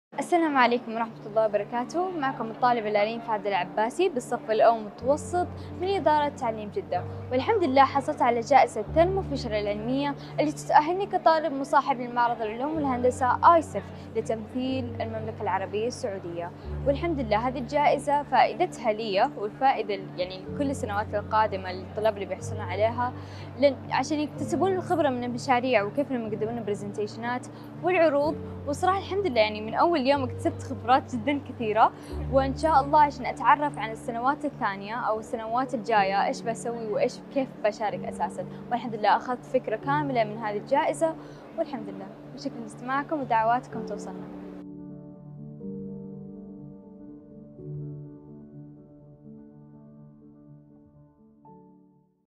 لارين العباسي - آيسف 2023
لارين العباسي..
السعودية الحاصلة على جائزة ثيرمو فيتشر، والتي تمنح لأصغر الباحثين في معرض ومسابقة آيسف_2023 والتي تتيح لها حضور المعرض لتأهيلها في المشاريع واكتساب الخبرات.